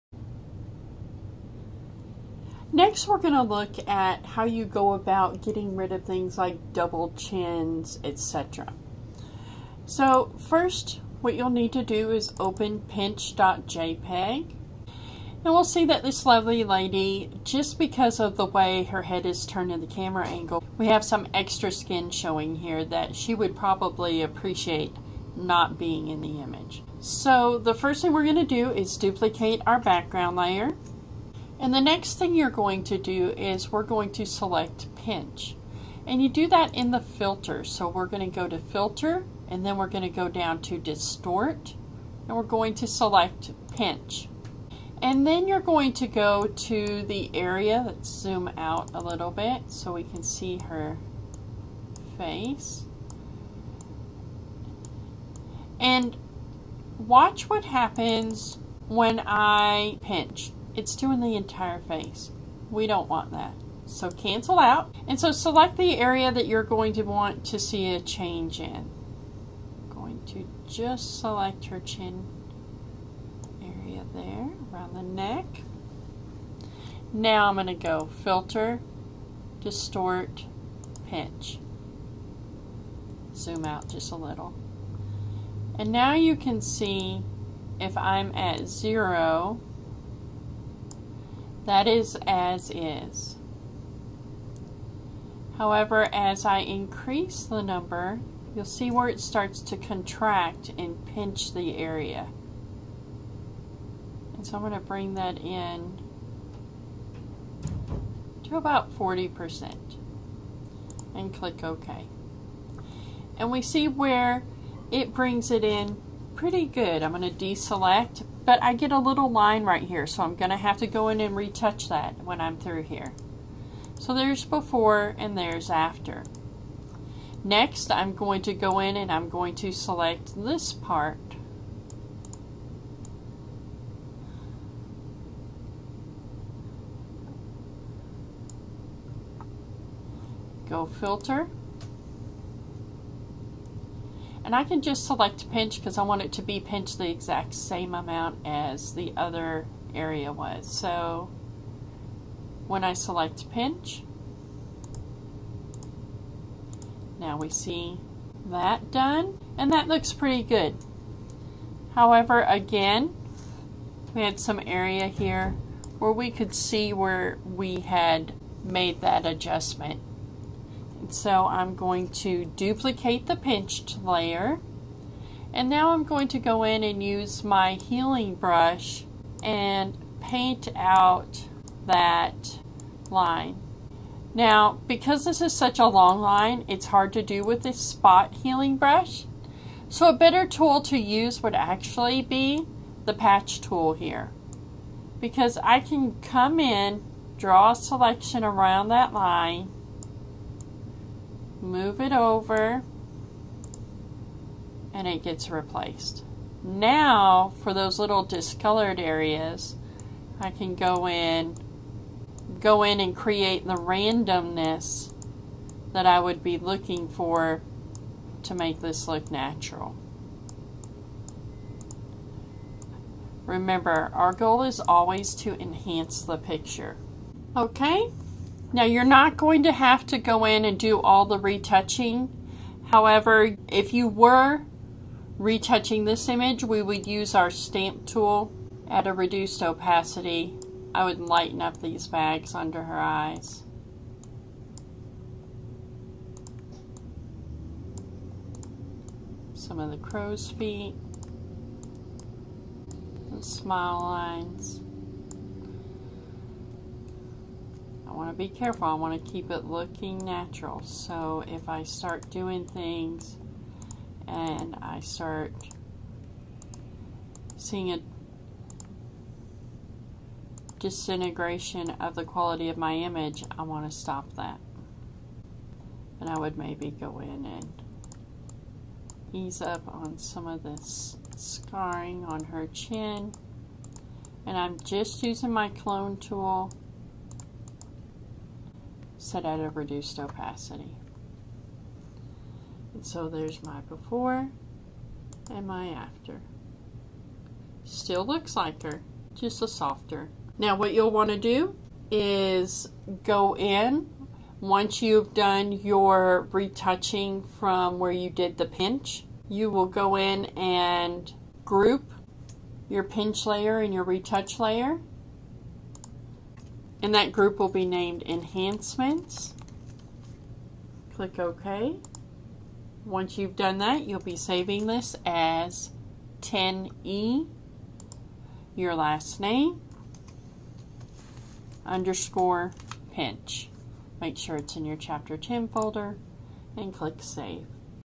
Photoshop Pinch 10E
How to use the Pinch filter in retouching a face